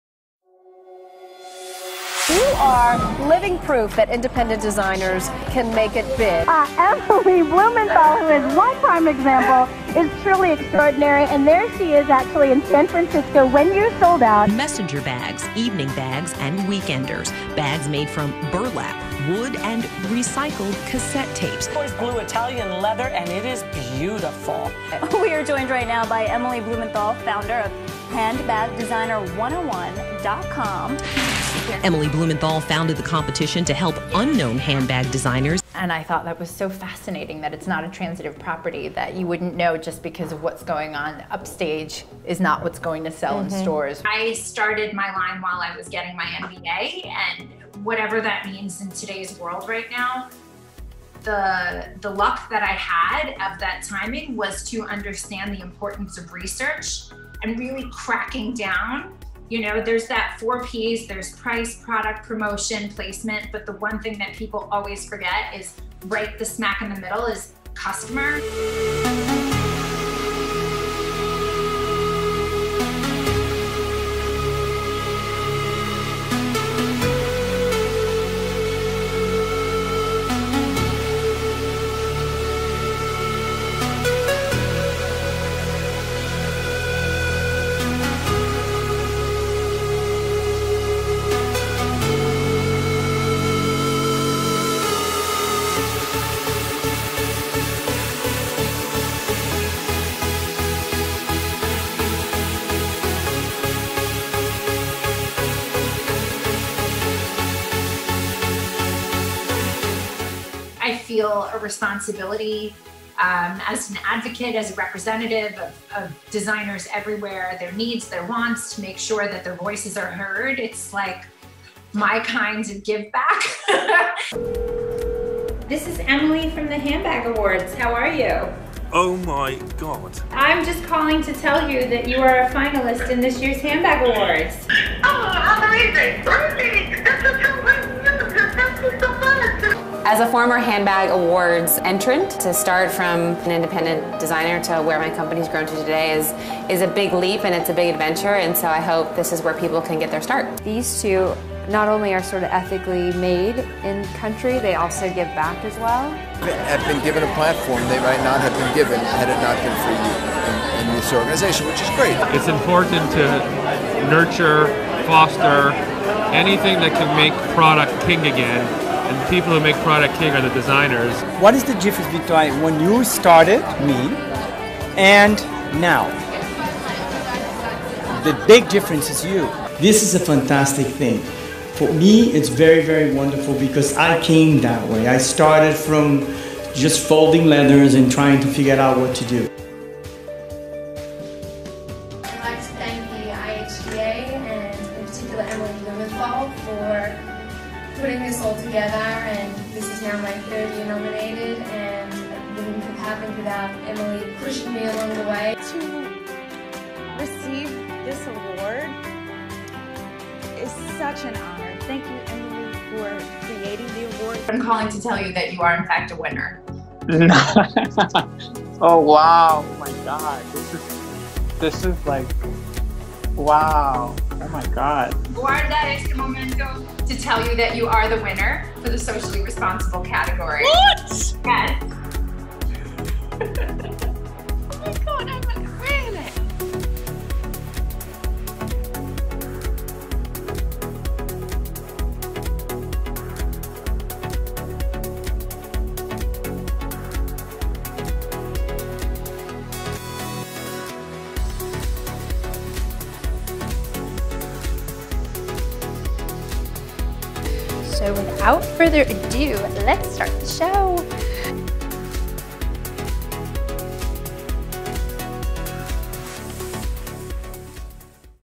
Emily Blumenthal: Handbag expert reel
Emily Blumenthal is the founder of Handbag Designer 101, Independent Handbag Designer Awards, and author of Handbag Designer 101: Everything You Need to Know About Designing, Making, and Marketing Handbags (sales worldwide over 50K copies).  As featured in The New York Times Style Section, Emily is known as the “Handbag Fairy Godmother” who brings unknown brands to the forefront. She is a trend/source expert and understands the insatiable appetite for emerging and undiscovered designers.

Emily is also a professor at the Fashion Institute of Technology, where she teaches entrepreneurship and was the first female hire for its Business Department. She has also taught “Fashion Marketing” at Parsons The New School for Design and “Fashion PR” at the Laboratory Institute of Merchandising. Emily has given lectures on “How to Launch Your Handbag Line” across the country and is the go-to for financial institutions on the handbag market overall and has been consulting for handbag brands big and small for over fifteen years.